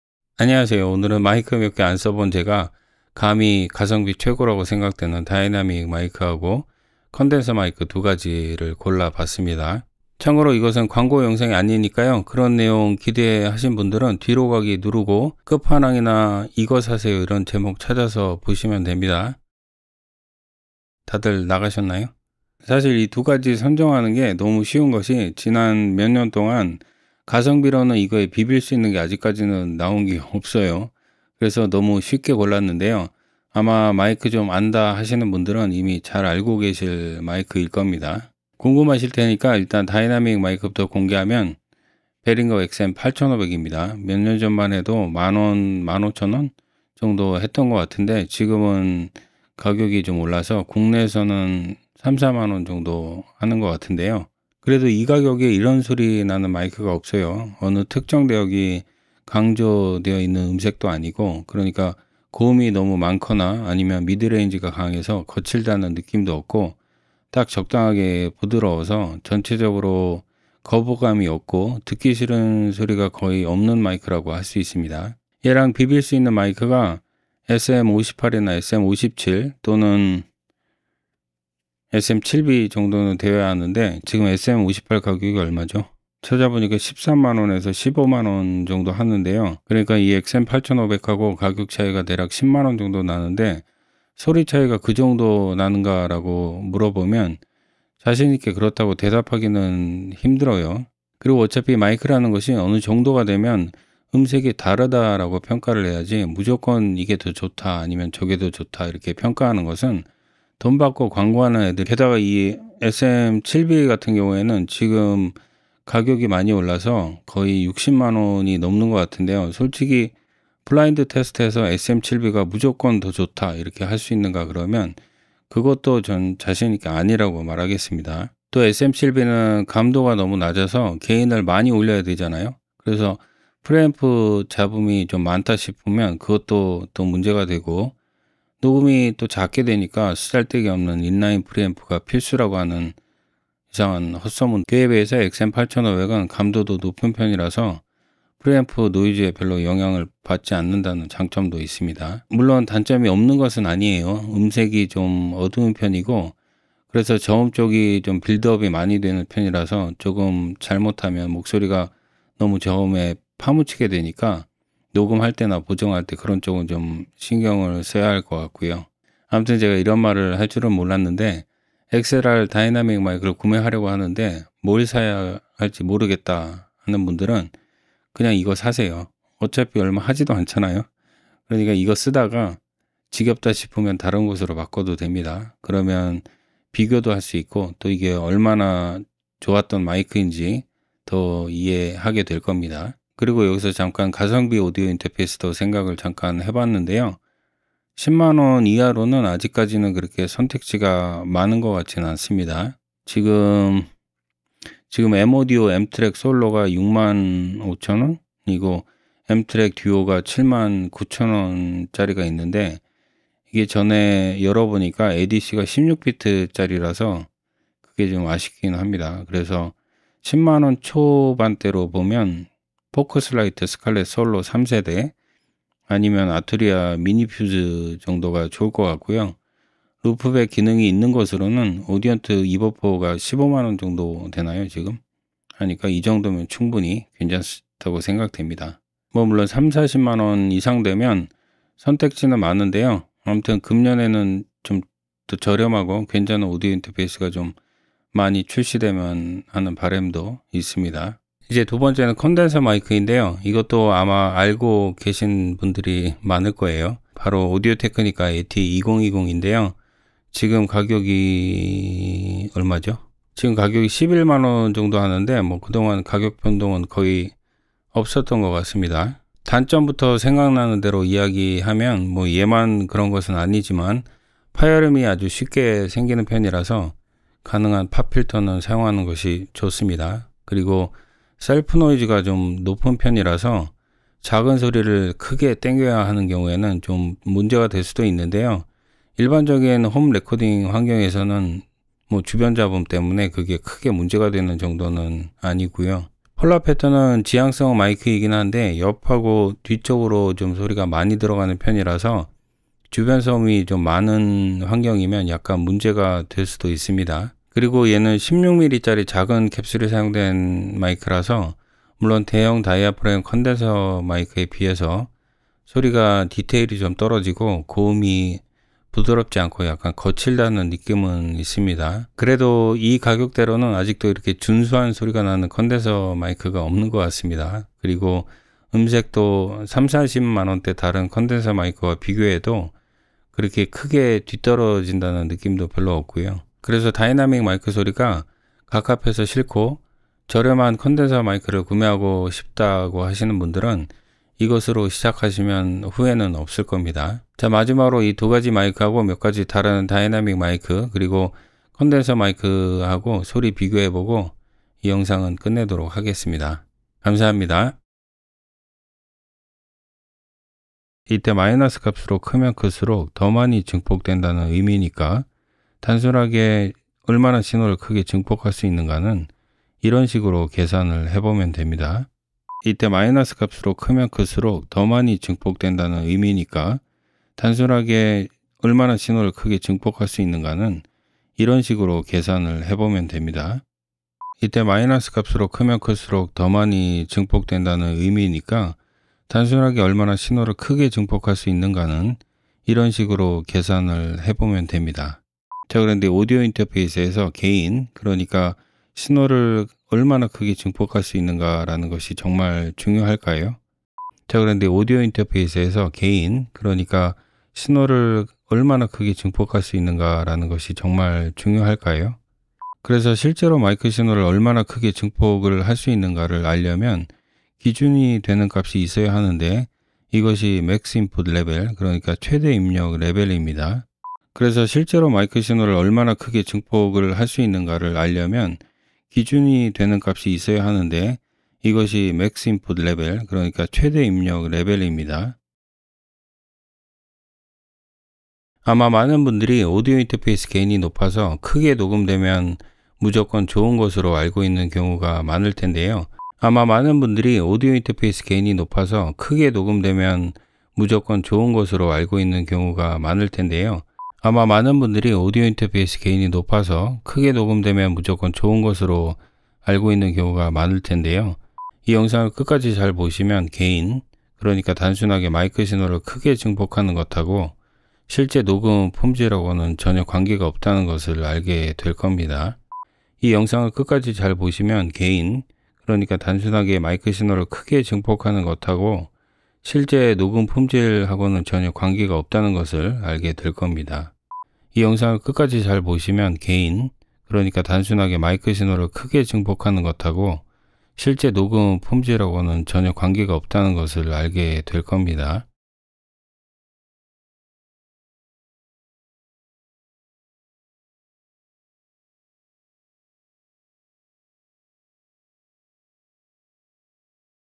가성비 최고 다이나믹/콘덴서 추천 마이크 (광고X)
가성비 추천 XLR 마이크(다이나믹 / 콘덴서), 오디오 인터페이스
영상에서 사용/추천한 장비는 본인이 구매한 것으로 제조사/판매처와 영상은 관계가 없습니다.

🦴 참고
- 영상에서 사용한 인터페이스: 미니퓨즈 2, 24bit/48kHz, 후보정(X)
- 오디오 레벨은 -16 LUFS로 노멀라이즈 했습니다.
- 유튜브 챕터와 자막을 지원합니다.

⌚️ 타임라인
00:00 소개
00:38 추천 다이나믹 마이크
03:12 추천 오디오 인터페이스
04:11 추천 콘덴서 마이크
05:57 다이나믹 마이크 비교
07:15 콘덴서 마이크 비교